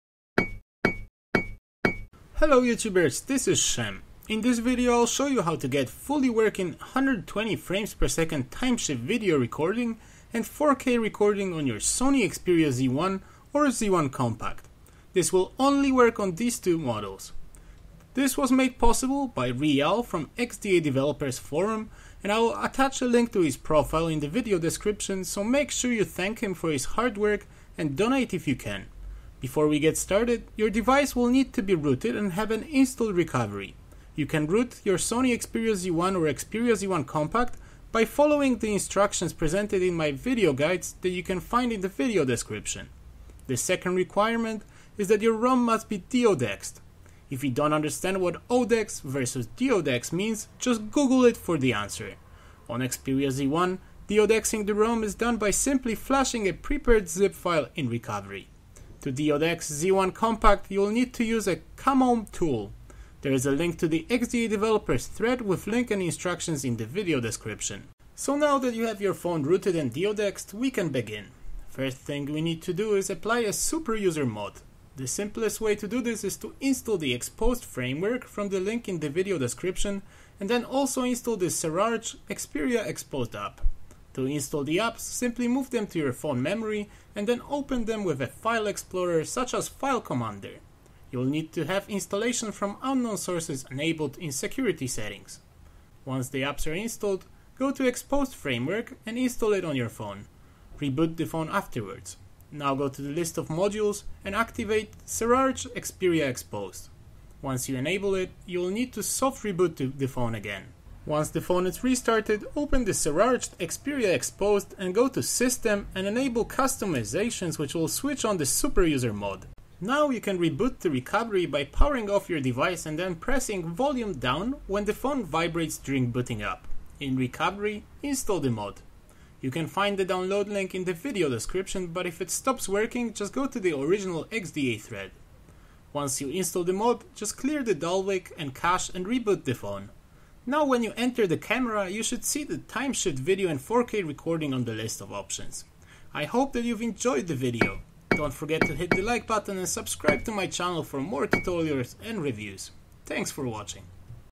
Sony Xperia Z1 and Z1 Compact 4K and 120fps Timeshift Video [ROOT]
Update: Deodex is NOT neccessary

READ ALL INSTRUCTIONS BELOW  !!!

I WILL NOT BE HELD RESPONSIBLE FOR ANY DAMAGE TO YOUR DEVICE
YOU DO IT AT YOUR OWN RISK!

YOUR PHONE MUST HAVE ROOT AND RECOVERY

THIS ROM WORKS ONLY ON ANDROID 4.4.2 KITKAT FIRMWARE

Deodex Xperia Z1 Compact by using this tool (instruction and download in the thread):

Necessary downloads:
1) Deodexing downloads (above)